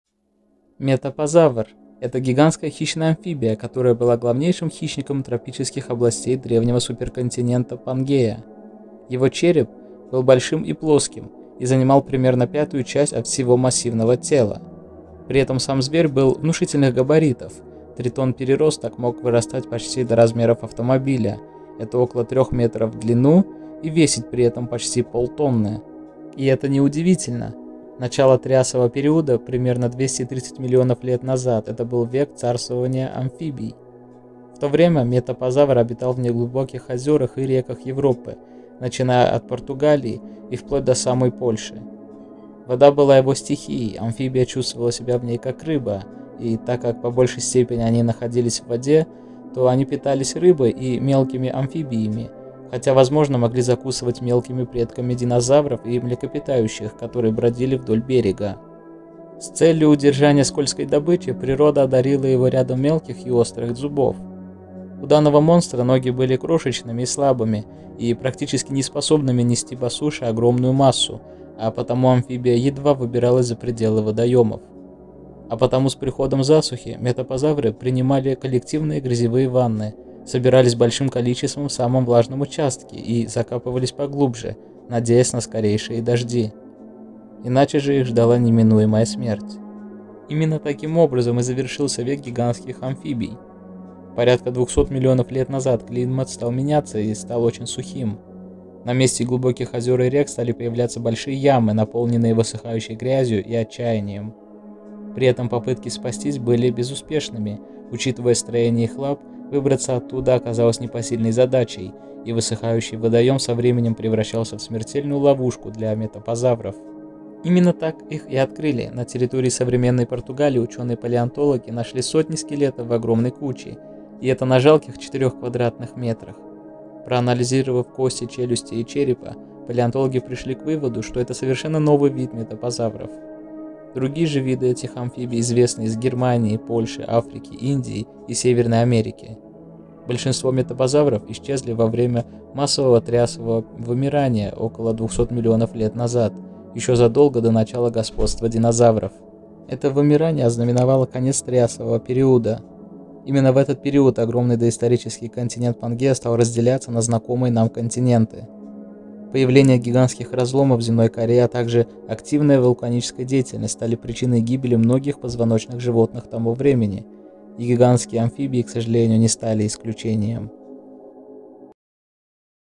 Метопозавр - хищная амфибия | Metoposaurus - predatory amphibian
Амфибии обитают как в воде, так и на суше. На сегодняшний день представителями амфибий являются саламандры, лягушки, тритоны и прочие, которые не славятся огромными размерами. Однако, в древние времена амфибии находились на вершине пищевой цепочки и были размером с автомобиль!